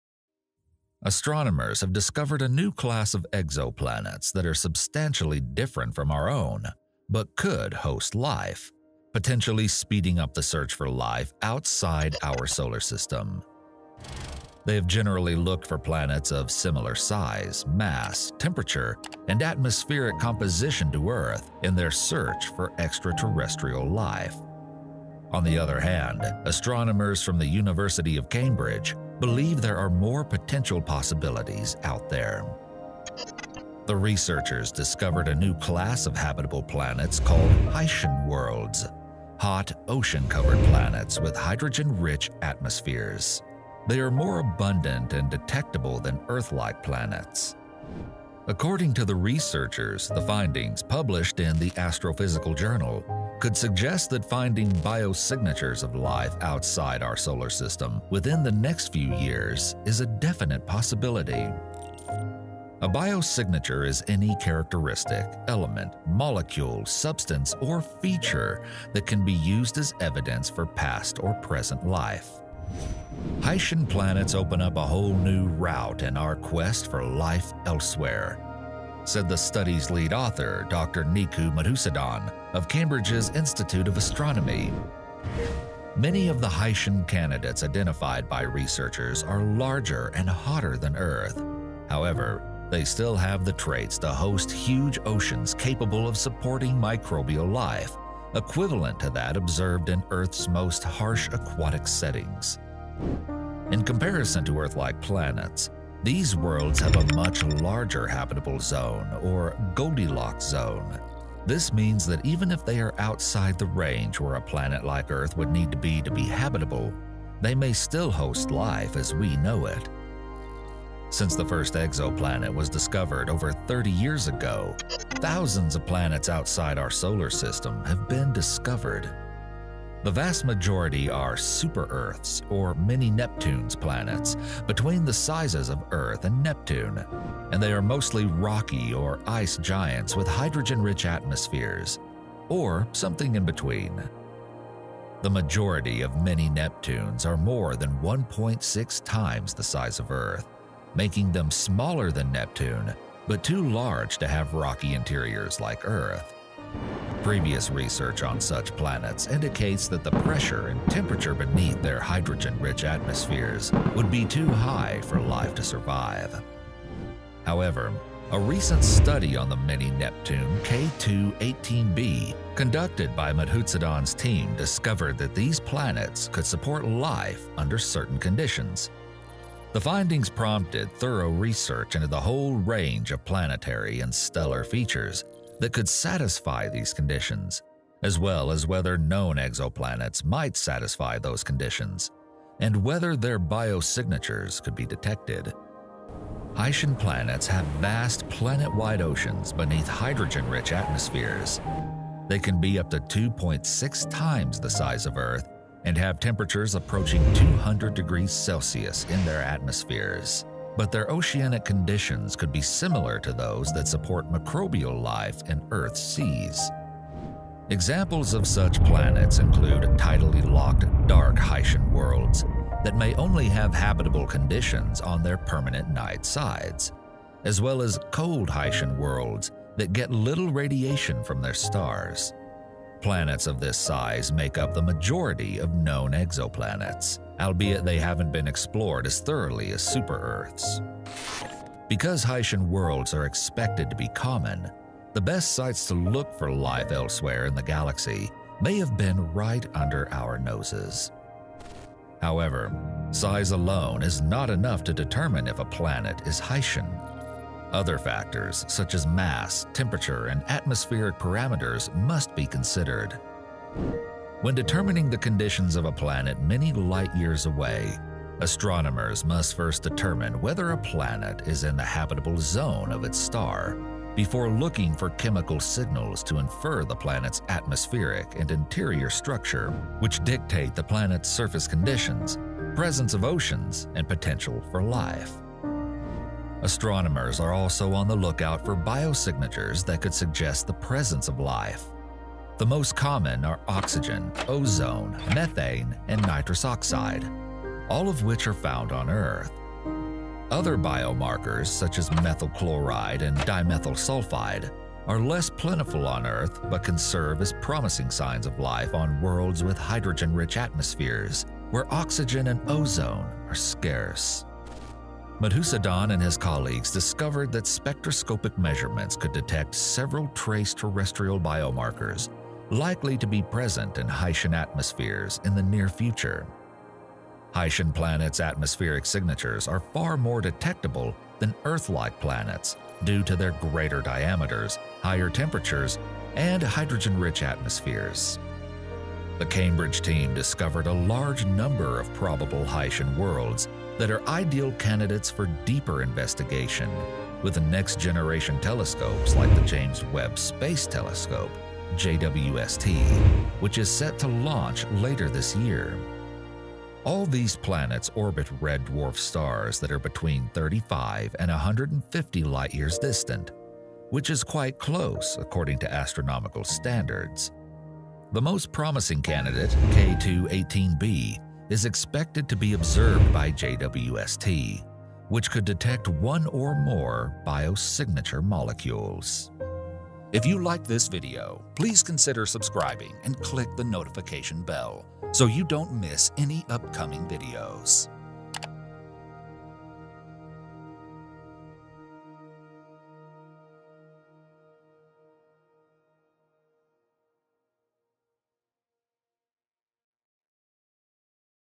Newfound Class of Planets Is Good News for Search of Life
The researchers discovered a new class of habitable planets called Hycean planets, hot, ocean-covered planets with hydrogen-rich atmospheres. They are more abundant and detectable than Earth-like planets. Hycean worlds are substantially different from our own but could host life, potentially speeding up the search for life outside our solar system.

Produced & Edited by:
Ardit Bicaj

Written by:
Nicole Amondi

Narrated by:
Russell Archey

Graphics:
Universe Sandbox 2
Space Engine

Thank you to our Patrons:
DonPig
xX-FLuKE-Xx